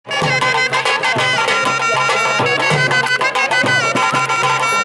Indian Wedding Music
These guys played at the wedding of our friends in Noida